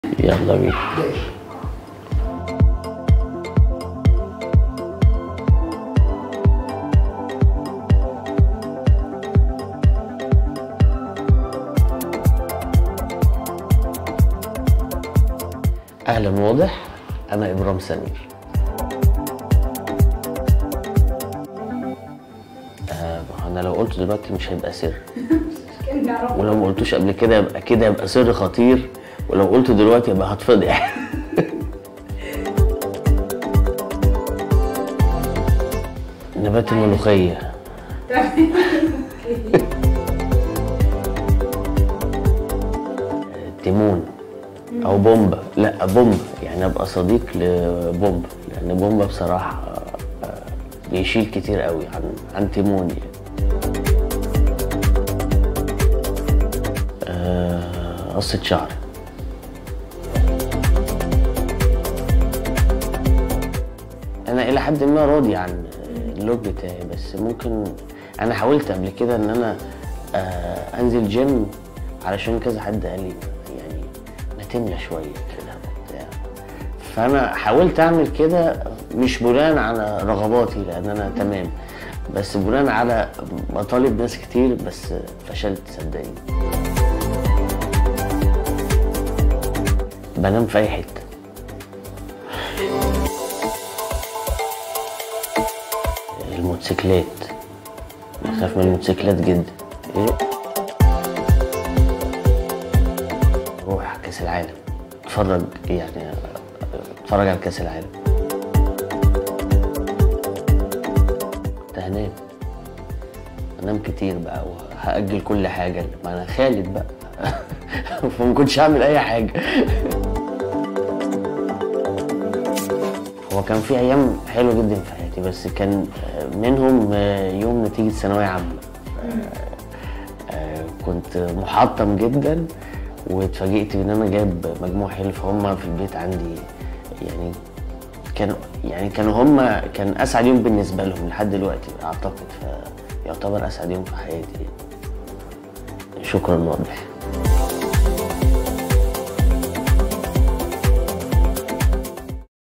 Ebraam Samir : Va-t-il nous dévoiler son secret ? 😂🤐
Wada7 rencontre Ebraam Samir, un jeune acteur égyptien et étudiant en médecin ! Va-t-il nous dévoiler son plus grand secret ? 😂🤐

Wada7 Original